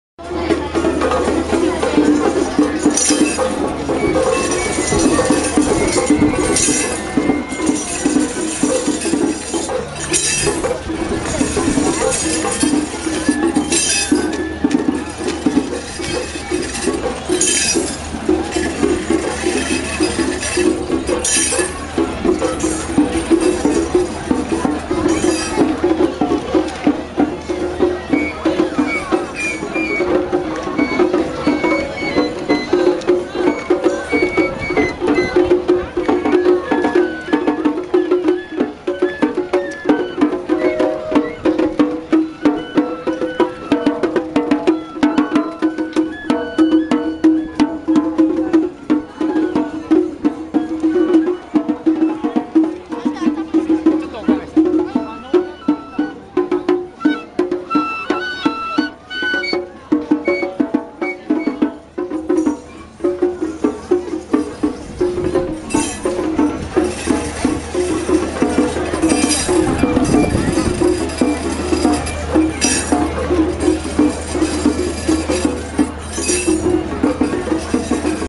第35回よこすかみこしパレード・米海軍基地内の様子・田之の浦
横須賀市・第35回よこすかみこしパレード・米海軍基地内（U.S. Fleet Activities Yokosuka）の様子・・・山車の様子（田之の浦・皆さん）綺麗な女性が花笠で（衣装は江戸時代かな）先頭で後に山車の様子
2011年10月16日